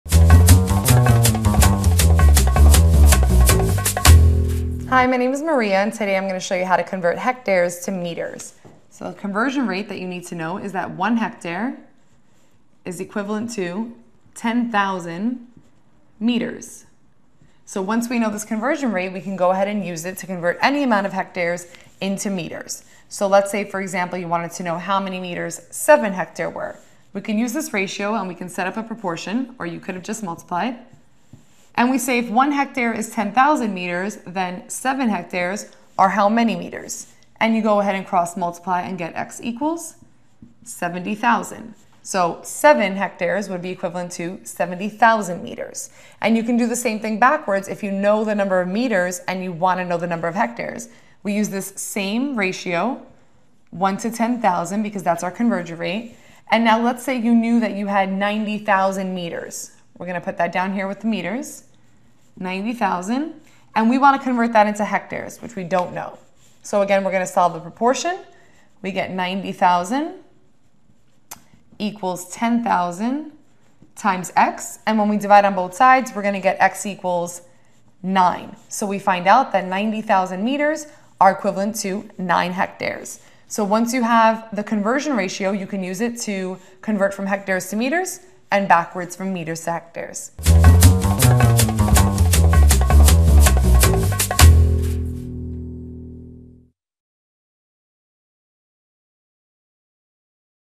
How to Convert Hectares to Meters
How to Convert Hectares to Meters. Part of the series: Math Education. Converting hectares to meters is a process that requires you to know how much of one equals the other. Convert hectares to meters with help from a mathematics educator in this free video clip.